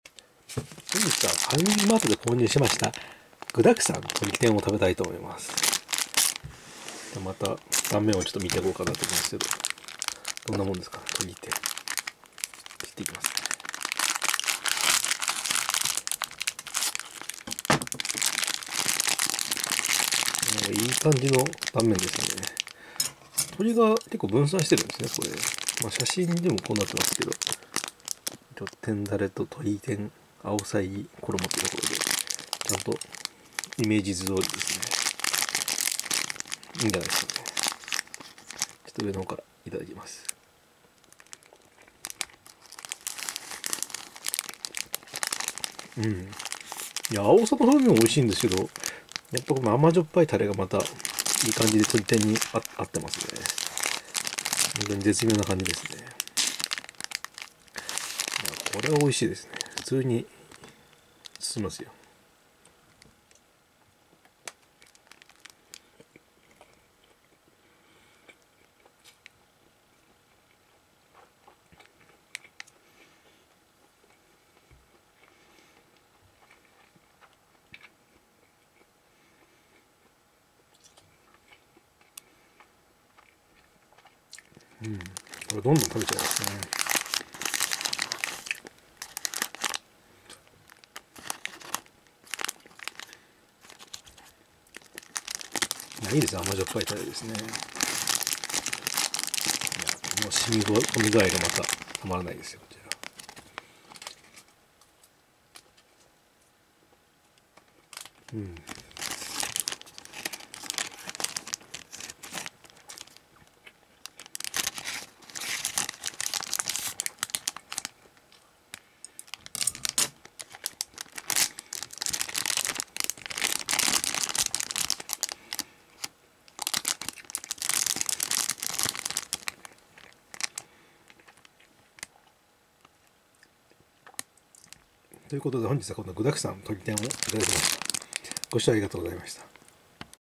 ファミリーマートの具だくさんなとり天のおにぎりを食べてみました。
I tried the Toriten Onigiri with lots of ingredients from Family Mart.